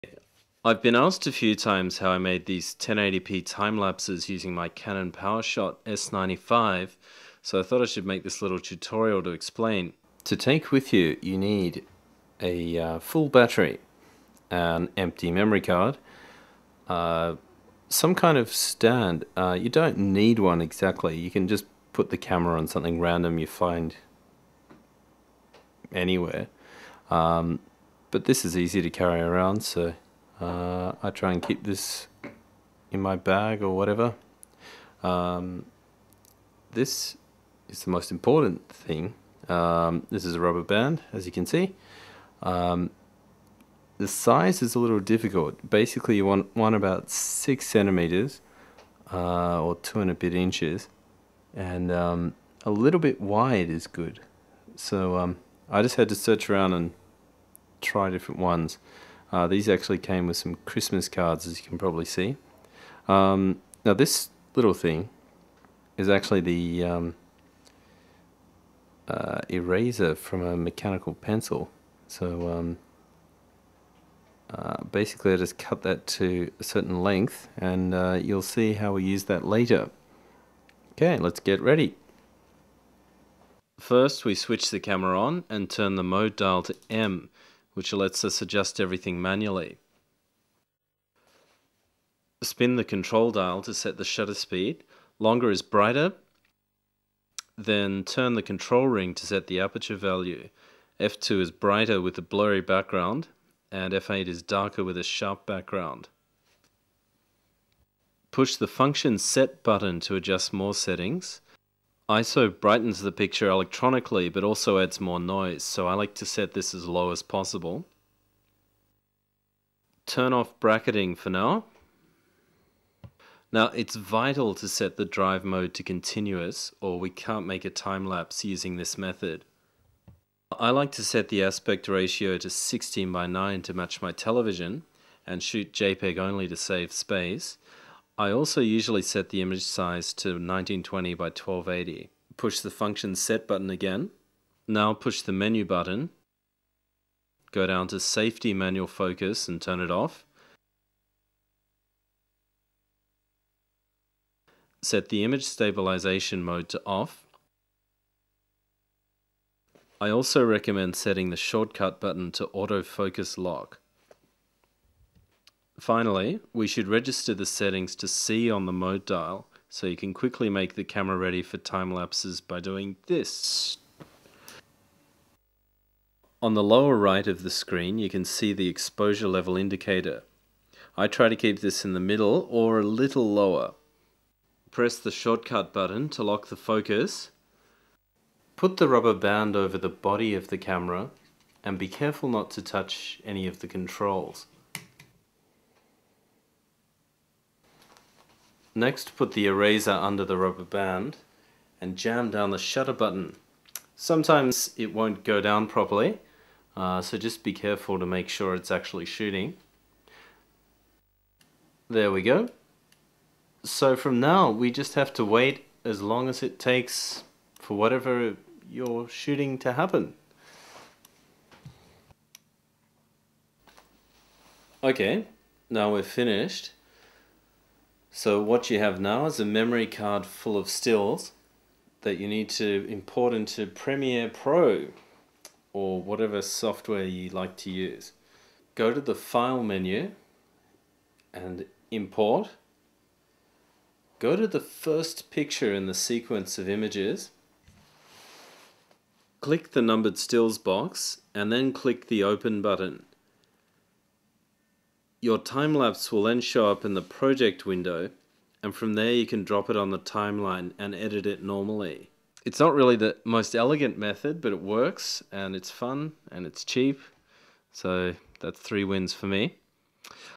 Canon PowerShot S95 1920x1080 Time Lapse Tutorial
A short tutorial (made before CHDK was available) on making high definition time lapses using the Canon PowerShot S95 camera, an eraser and a rubber band!